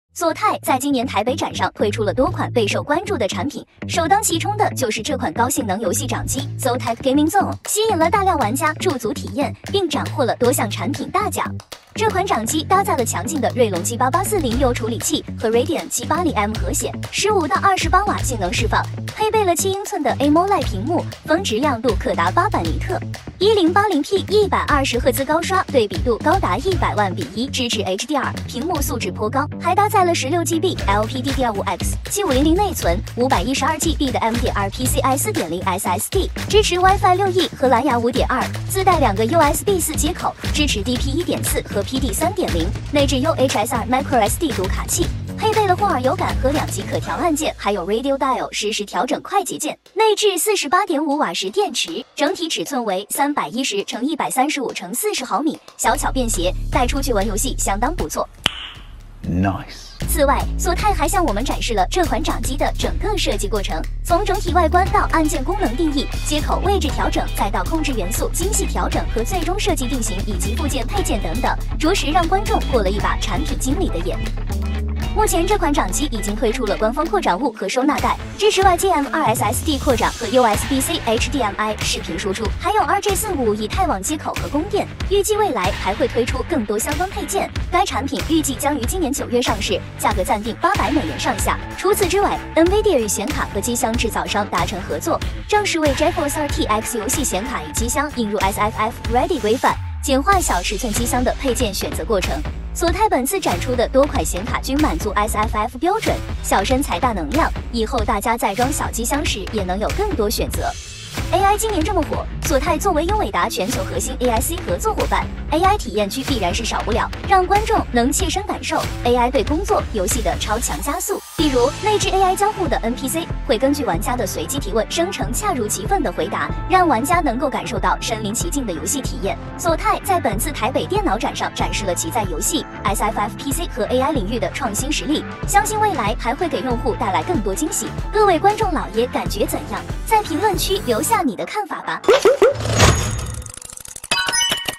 索泰台北電腦展發表高效能遊戲掌機，還有RTX SFF顯示卡和AI體驗！
來看看索泰在台北電腦展都發佈了啥？